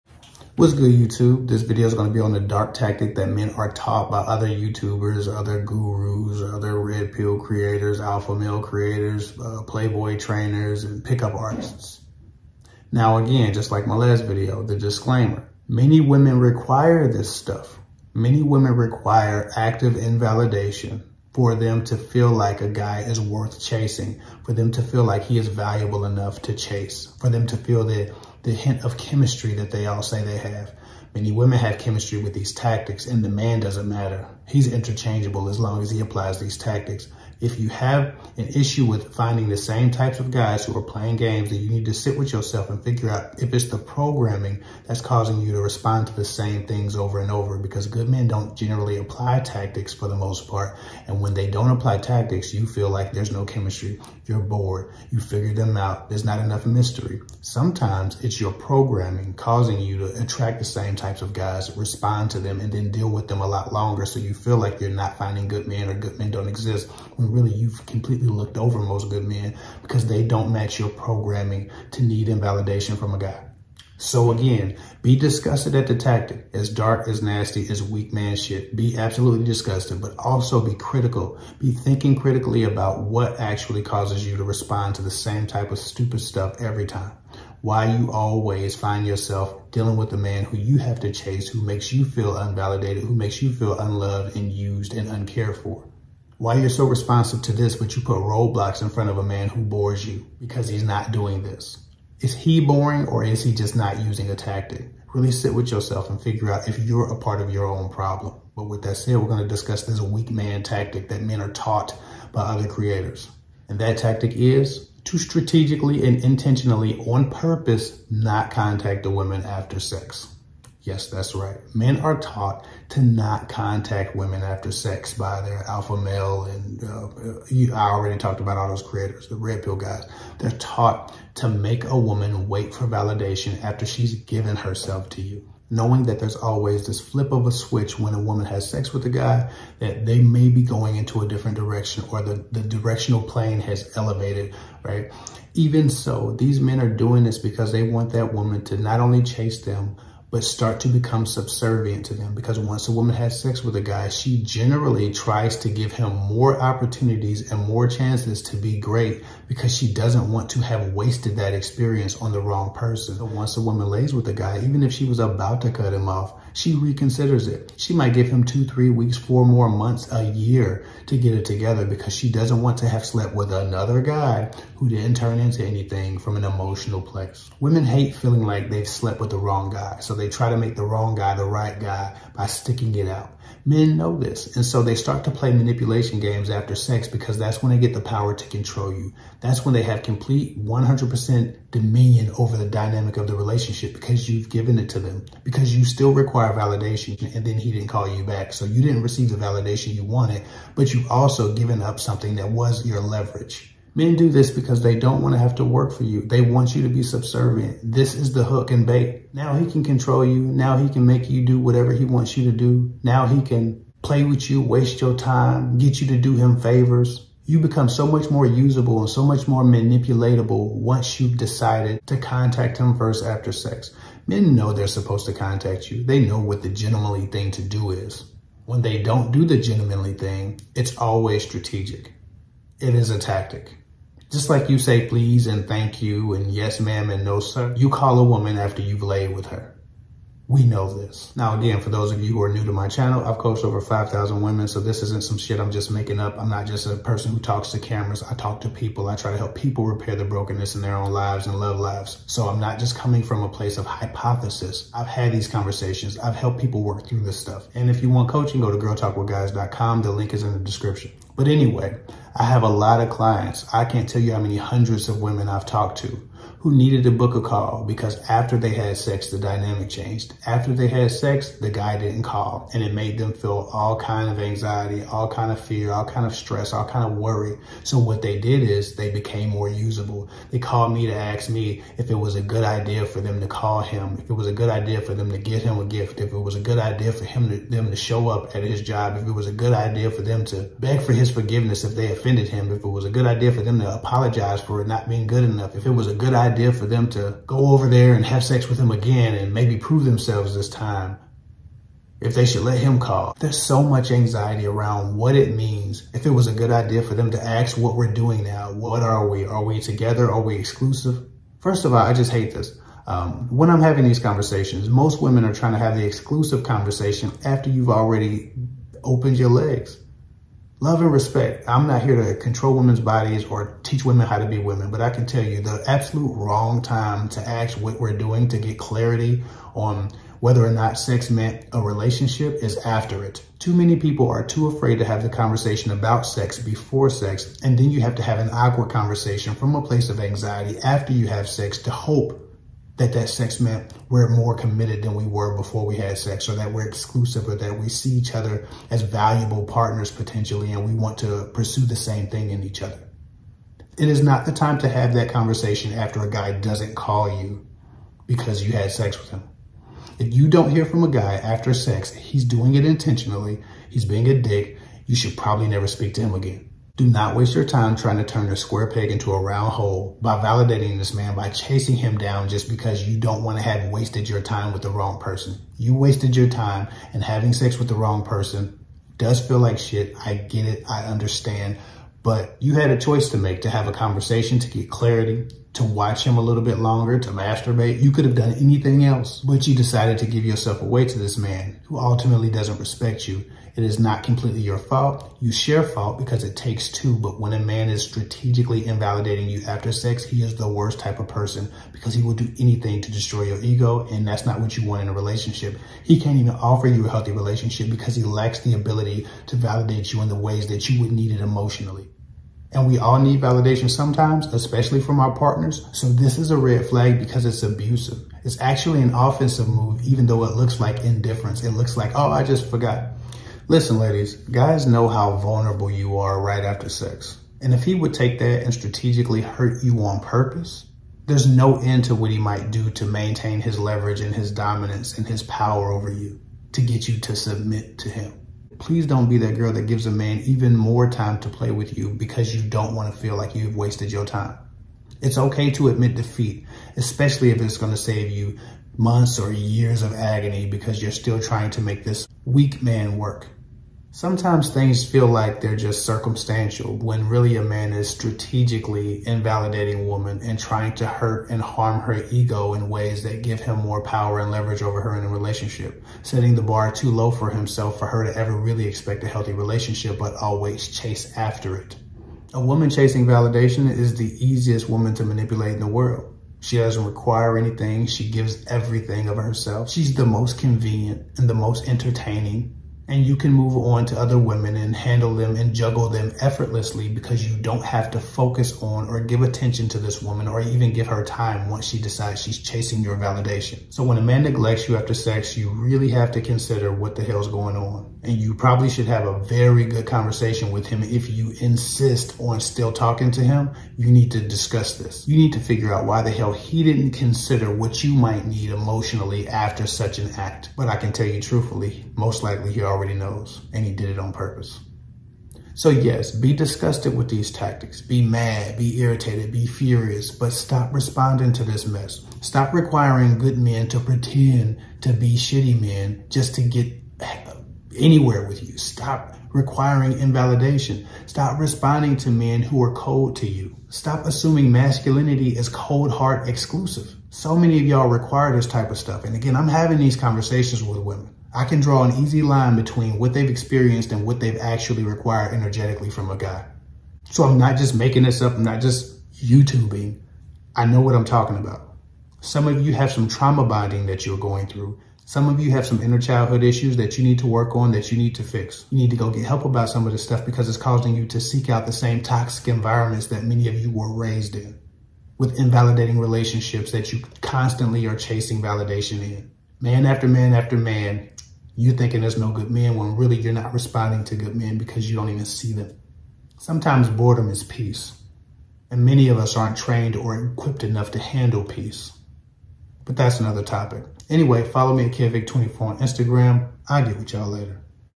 EVIL MANIPULATION TACTIC MEN USE to CONTROL WOMEN | Dating mistakes women make 🚨
Girltalkwithguys.com

Part one if this series can be found here-
Sneaky Trick guys use on women to hurt their ego | Dating red flags 🚩

Guys often use sneaky manipulation tactics and tricks to get women to chase them. It’s meant to destroy their ego, and set the stage for a pump and dump. Sometimes guys are playing a sick game of tag. Too many women play along and play themselves in the process. Don’t respond to any guy who tries to get you to call him after flirting with you or sleeping with you.

Pick up artist tactics
Manipulation games guys play
Sign he wants you to chase him